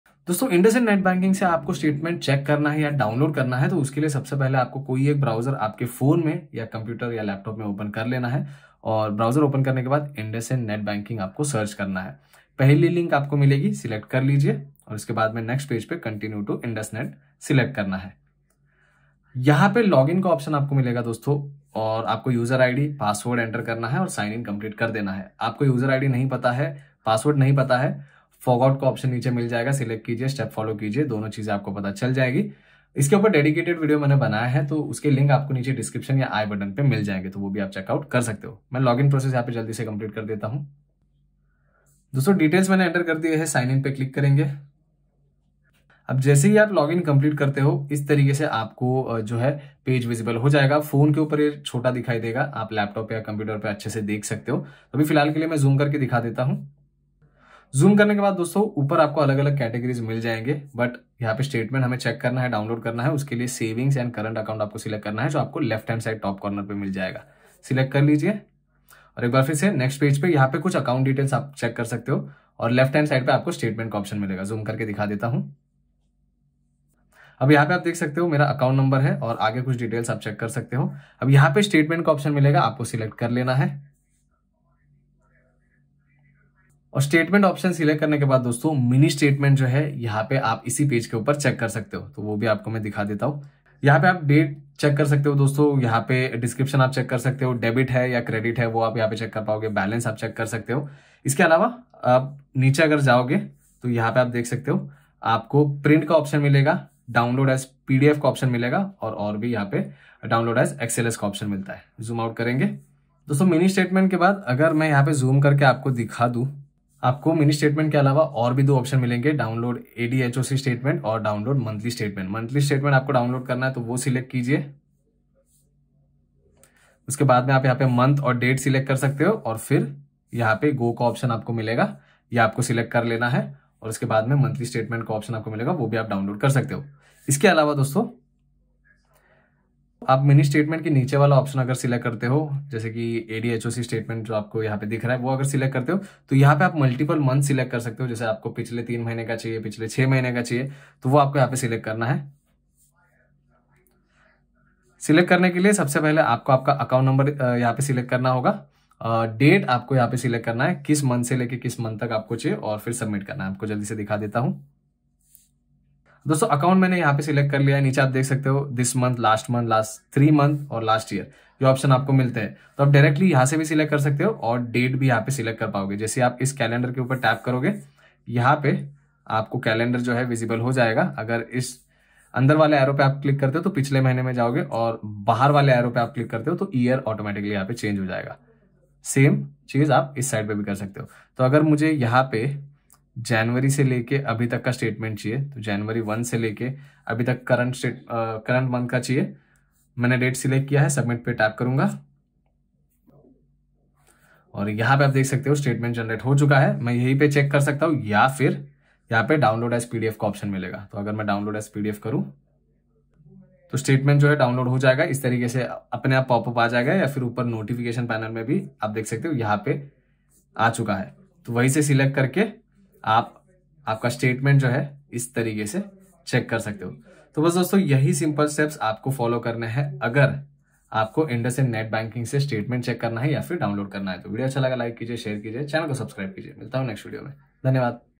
IndusInd Bank Ka Statement Kaise Nikale? How to Download Net Banking Statement of IndusInd Bank
How to download statement in IndusInd bank? If you know the net banking credentials of your IndusInd bank account, you can easily download the statement of a specific time period in the IndusInd net banking portal. Here is a brief tutorial that will teach you how to download the IndusInd bank net banking statement.